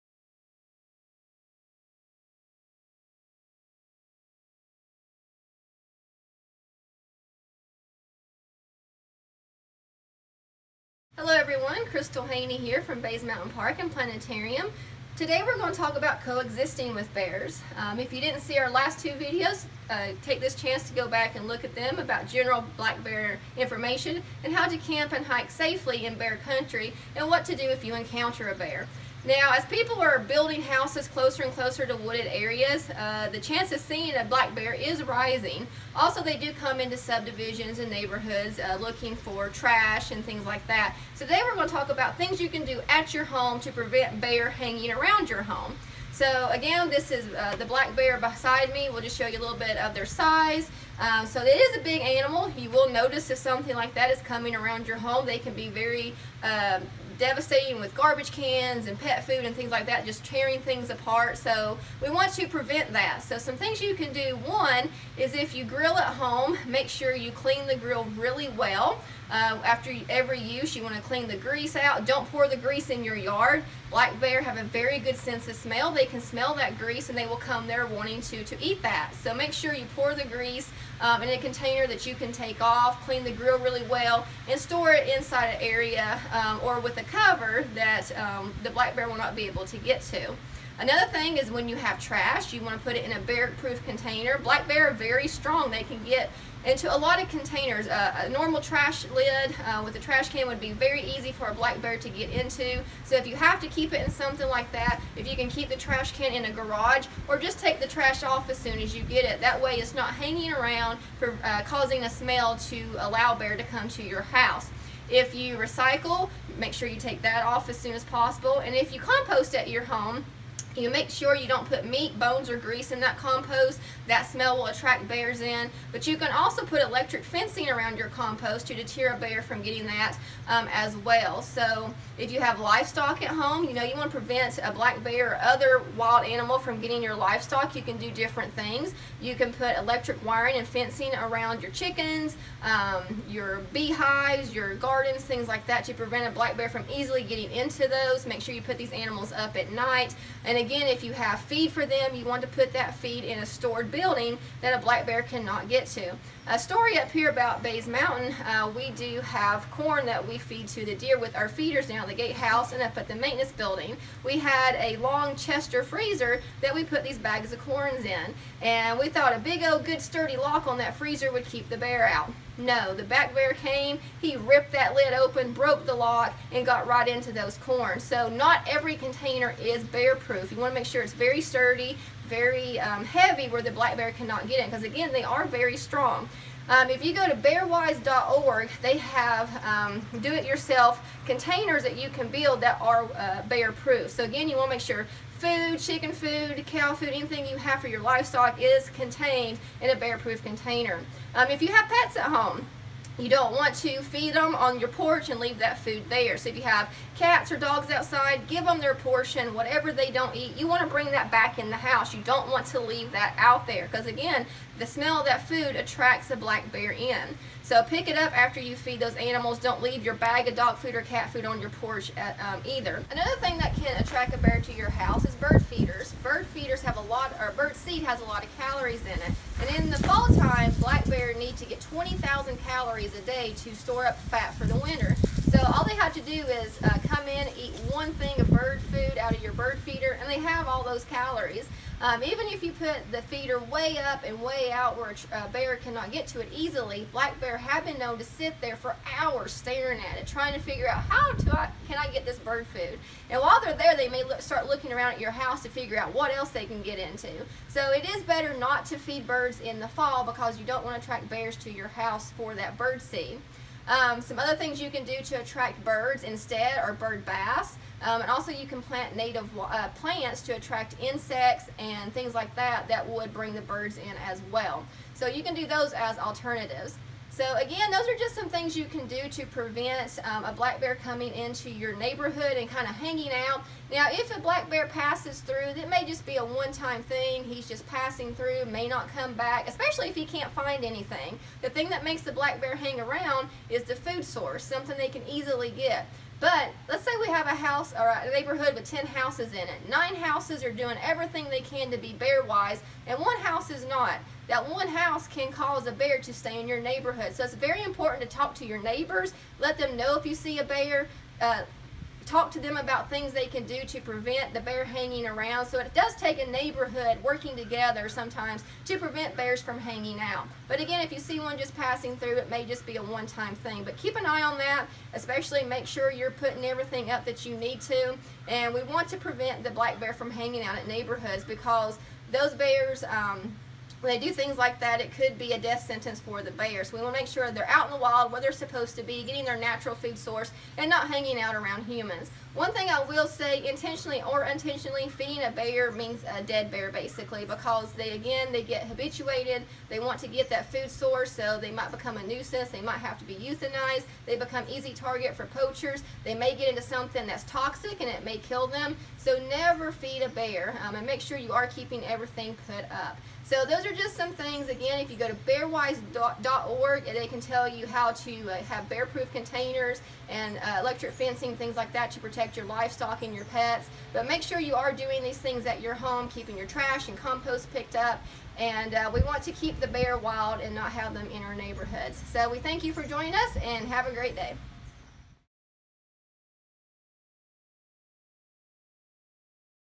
BMP - Bear Aware - 3 - Coexisting with Black Bears - 2021
Learn all about how to safely coexist with the black bear!